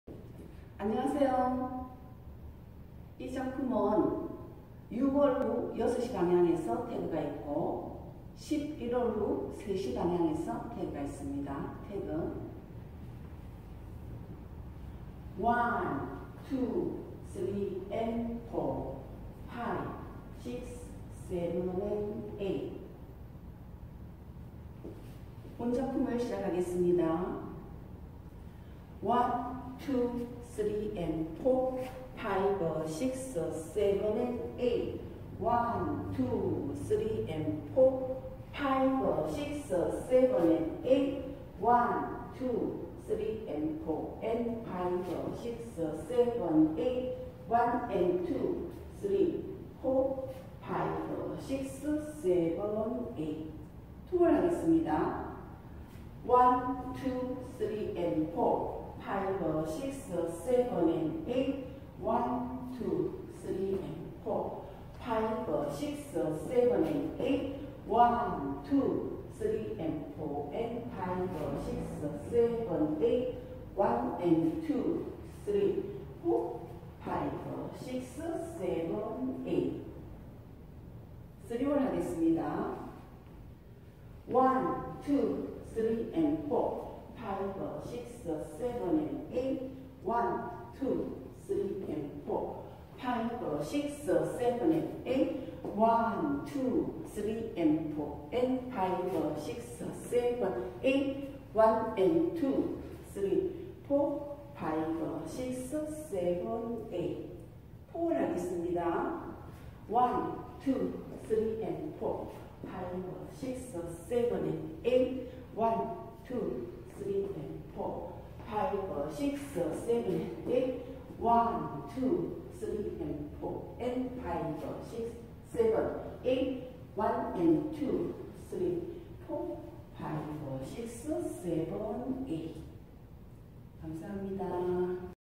Chichiquita Cha Cha Linedance /Improver (COUNT)
32c, 4w, Improver
Tag (8c) : After 6w (6:00) & 11w (3:00)
Ending: End of Wall 13, Turn 1/4 Right(12:00)+Tag
Choreo: Regina Cheung & Ping Chen (January 2021)